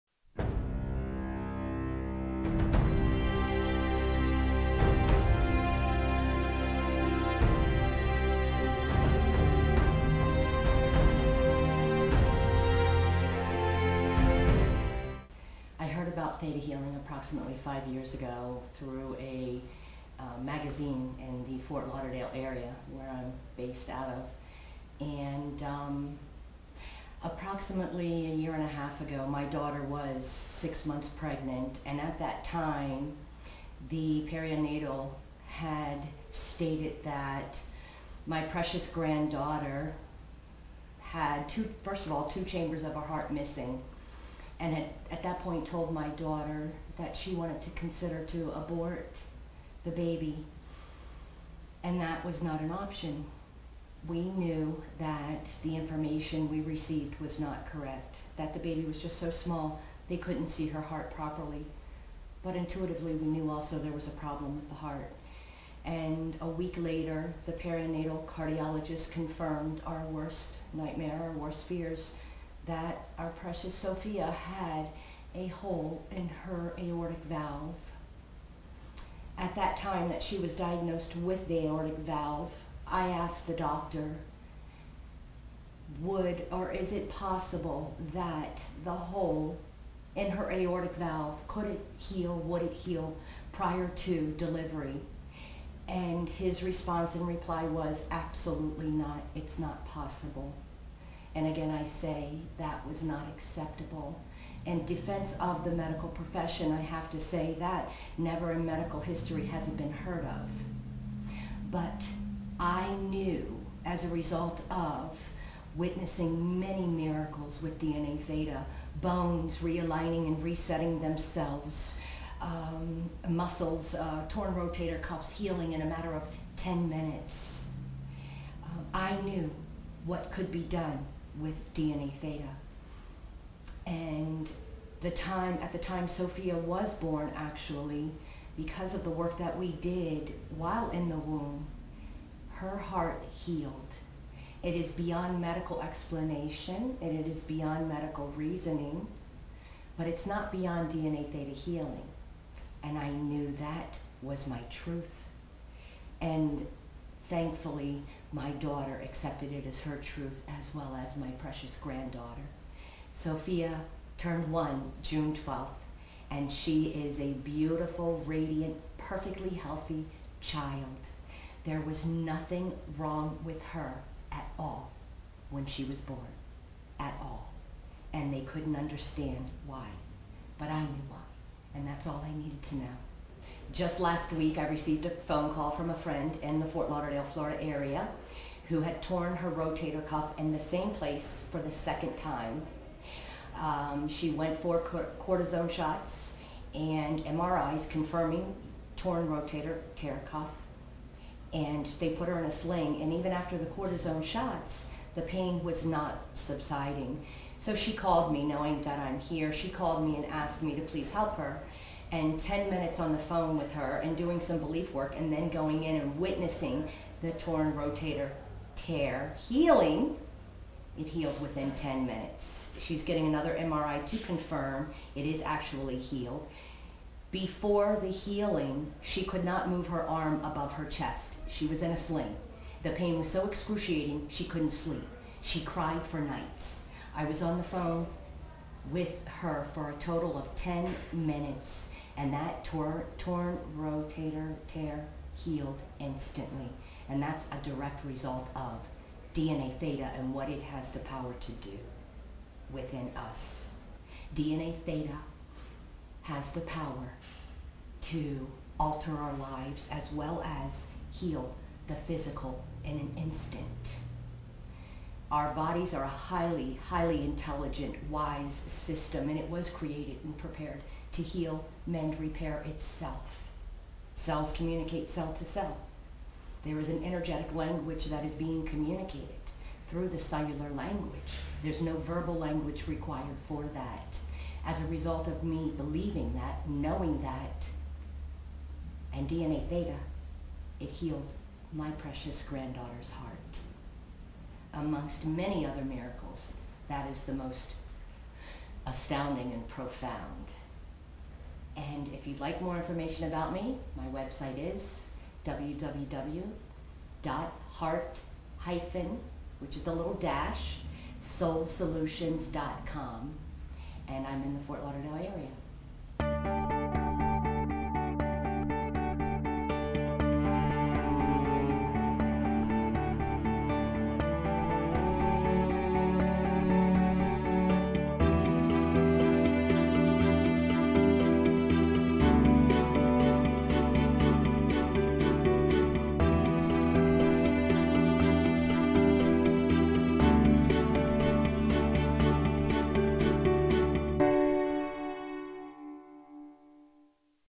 ThetaHeaing Testimonial by April Colon Pagan.
April Colon Pagan gives her ThetaHealing Testimonial at the THInK Institute earlier this summer.